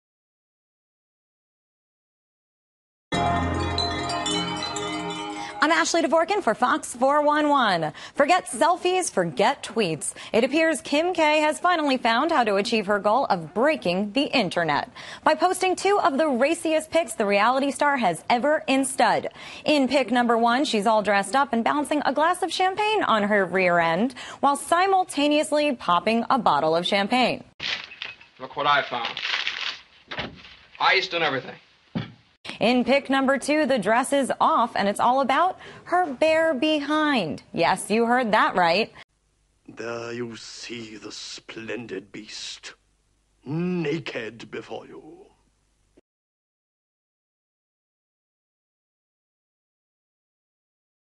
WHAT KIND OF DEAL DID SHE GET ON THE BAD END?
Kim Kardashian pose nude for Paper Magazine.